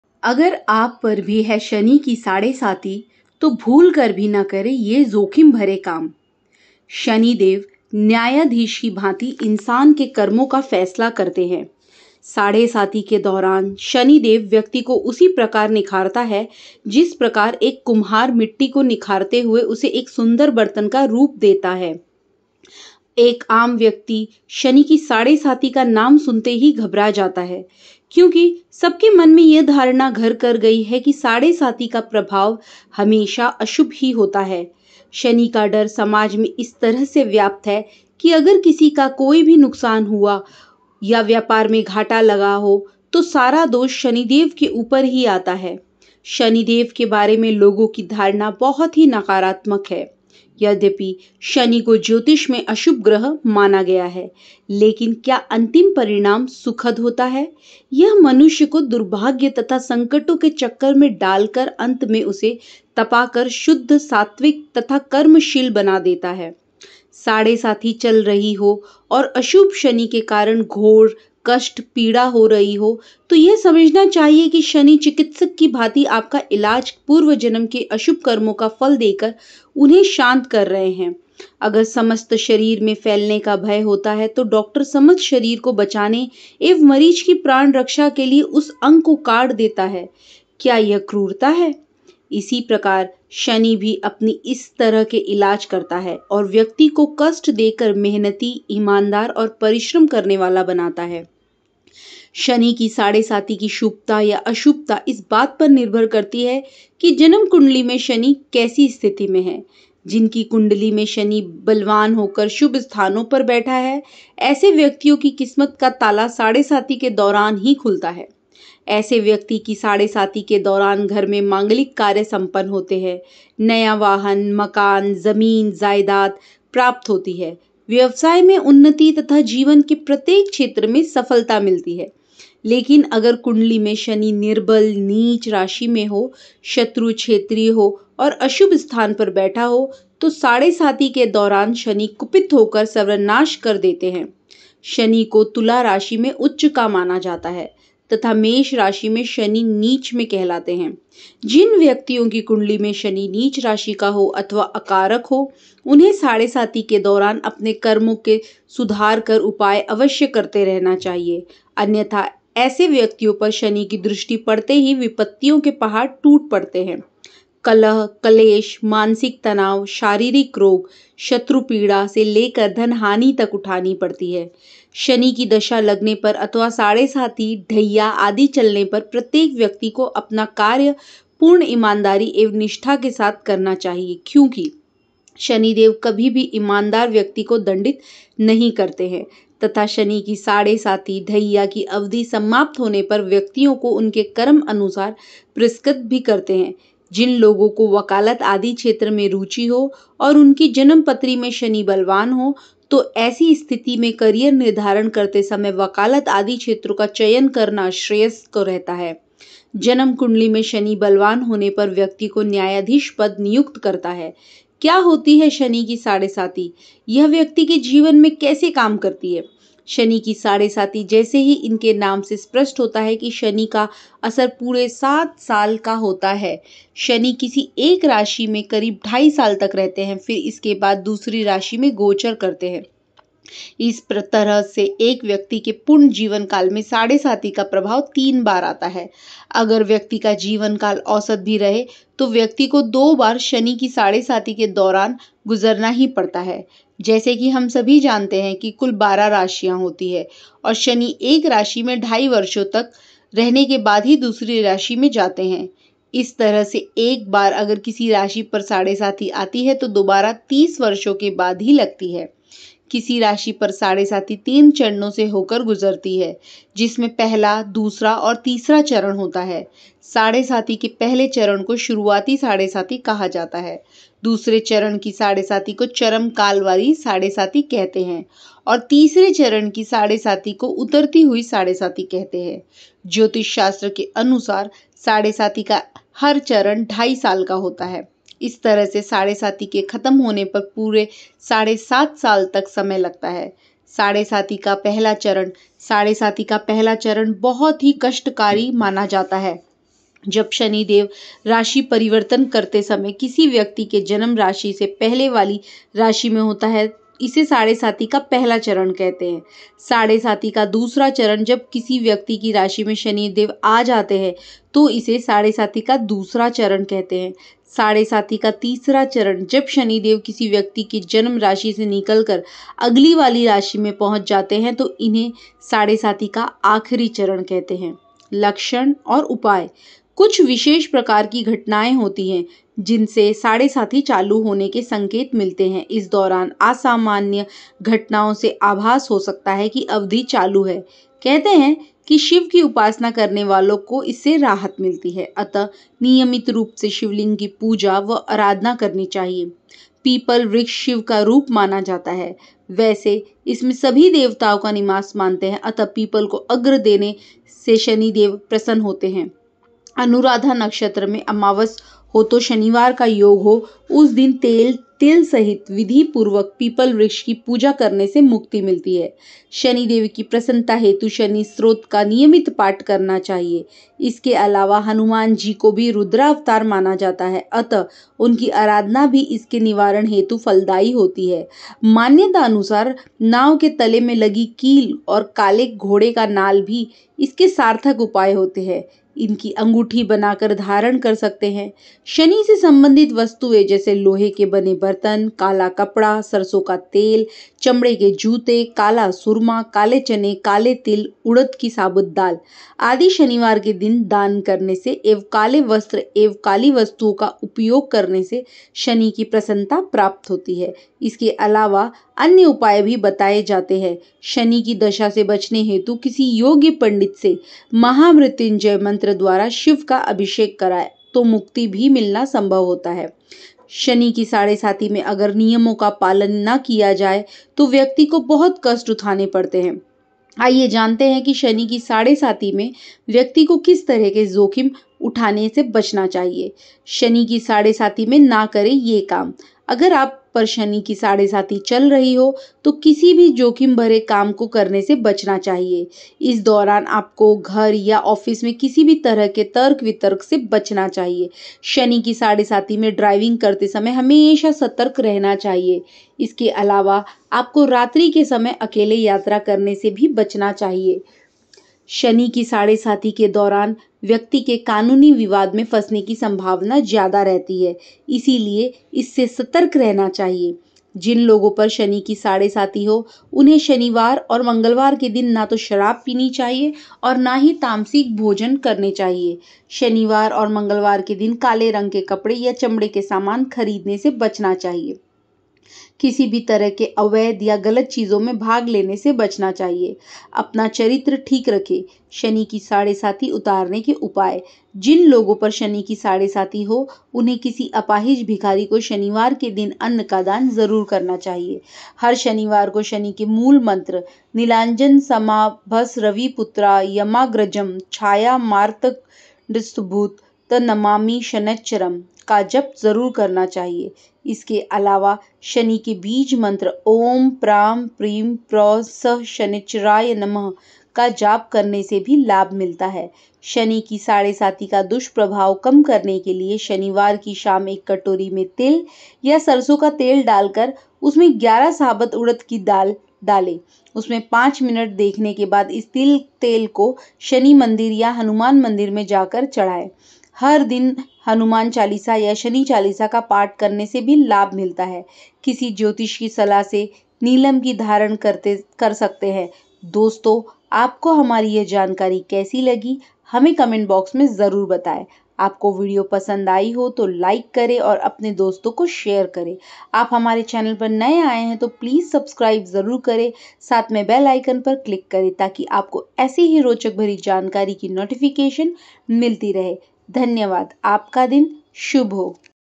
शनि की साढ़ेसाती है तो भूलकर भी ना करें ये जोखिम भरे काम ।साढ़ेसाती उतारने के उपाय
अगर आप पर भी है शनि की साढ़ेसाती है तो भूलकर भी ना करें ये जोखिम भरे काम ।साढ़ेसाती उतारने के उपाय |